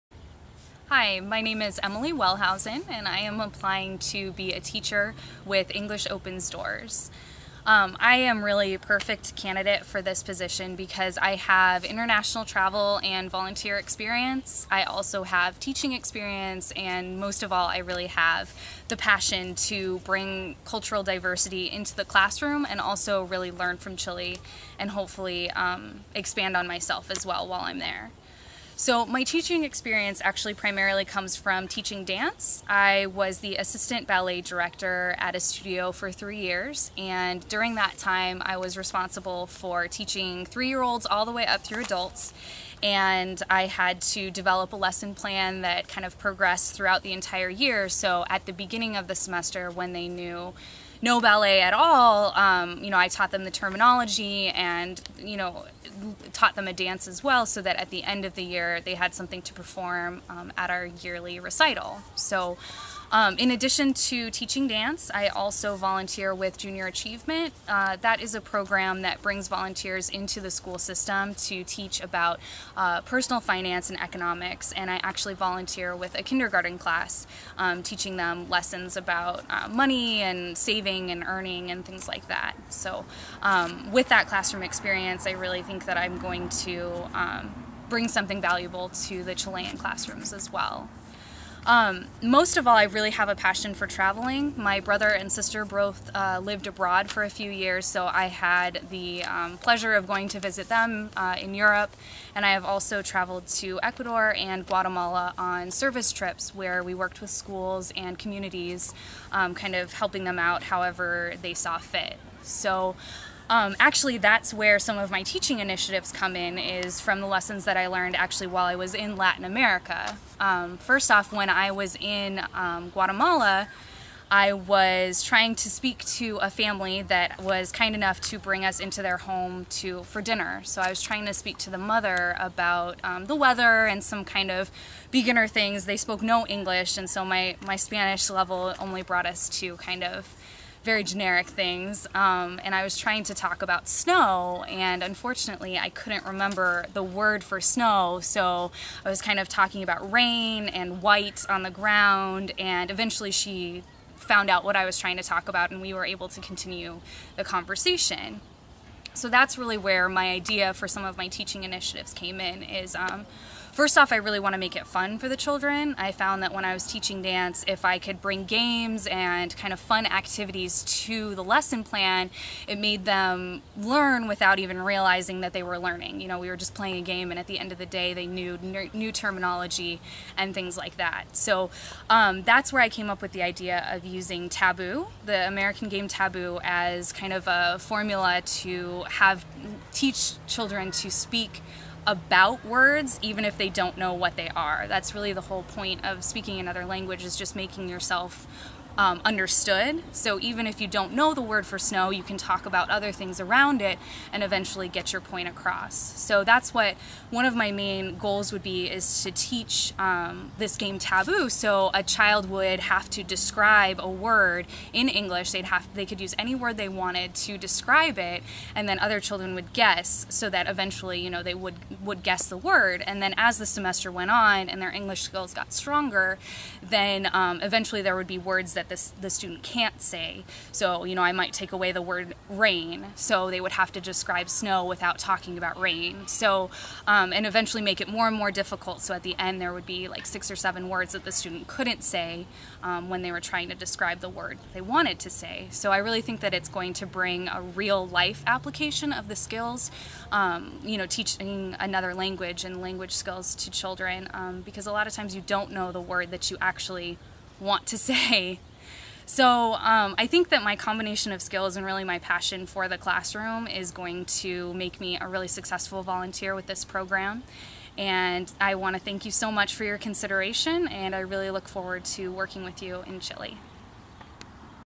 Emily Wellhausen Introduction Video
Video for English Opens Doors volunteer teaching position in Chile for March 2012.